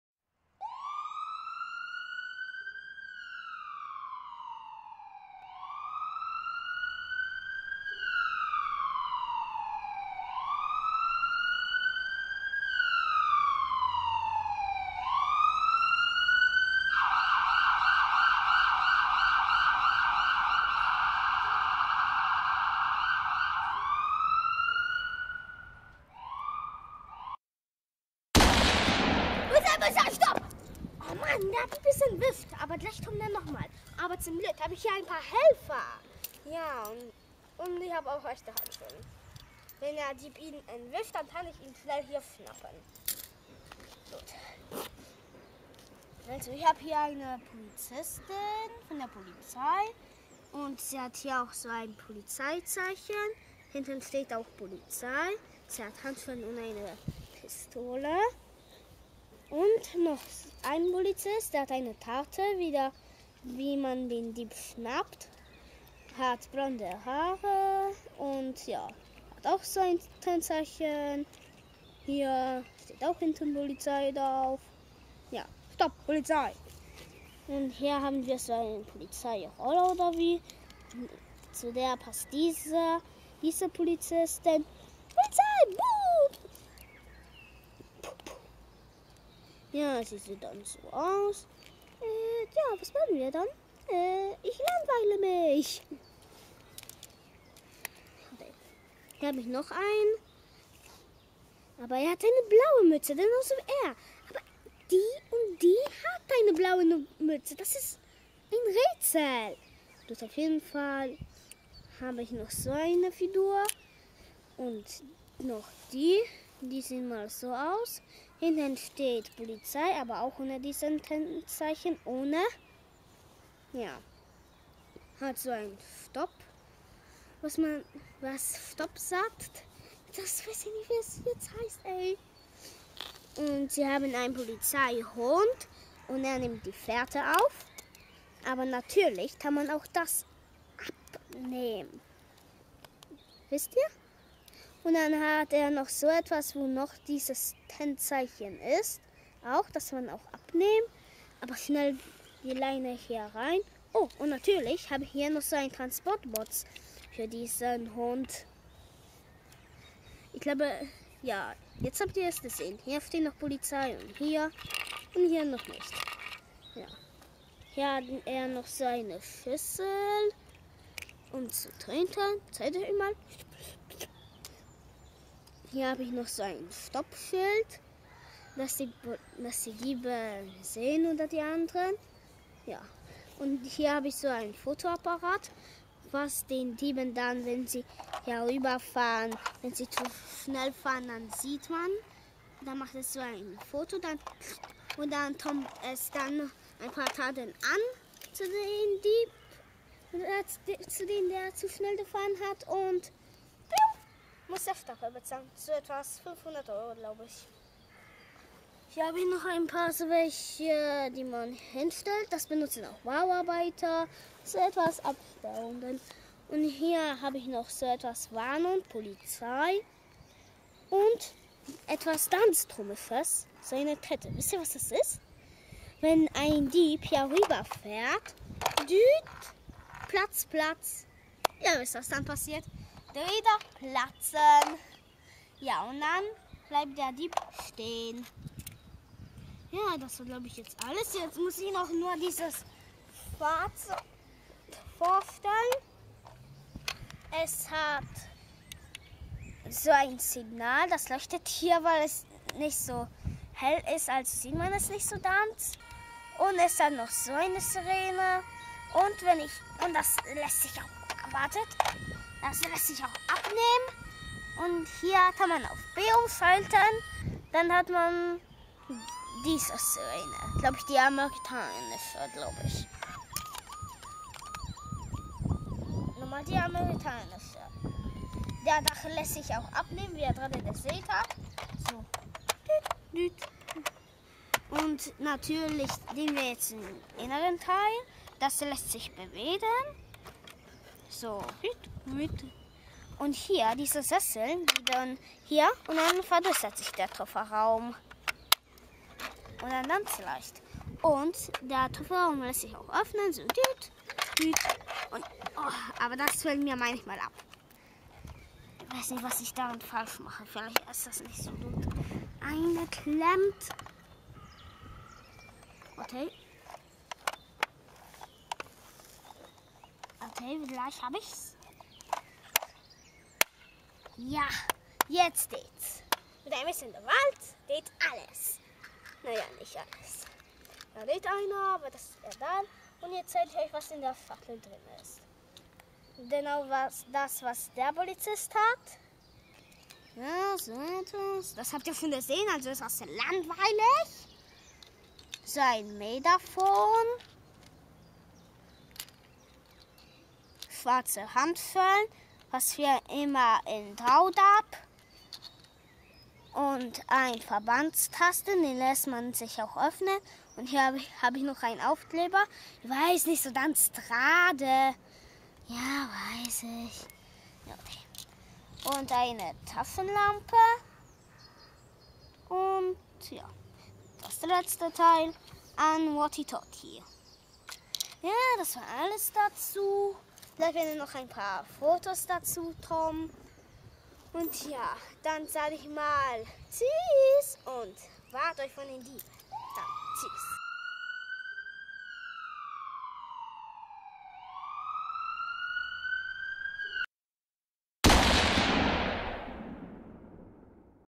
PLAYMOBIL Polizei Neucheit 2016: Polizeiauto; Straßensperre; Polizistin mit Balance Racer;
Playmobil Polizei Neuheit 2016:

Polizeiauto 6873
Polizei - Straßensperre 6878
Polizistin mit Balance Racer  6877